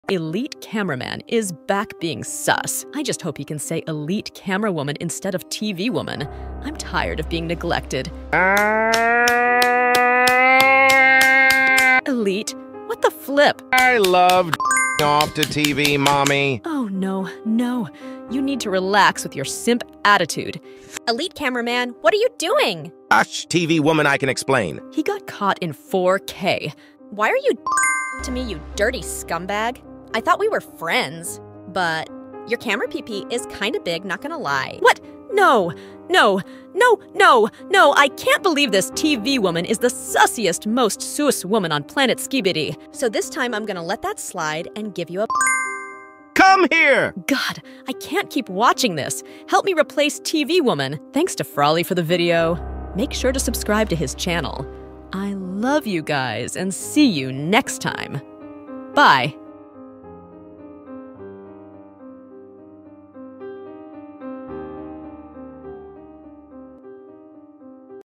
Reacting to Elite Cameraman Caught In 4k Kissing Tv Woman 💔 sus
Elite camerawoman Here!

tv woman caught elite cameraman! but i still love him, one day i will marry him.

credits: @NotFrlly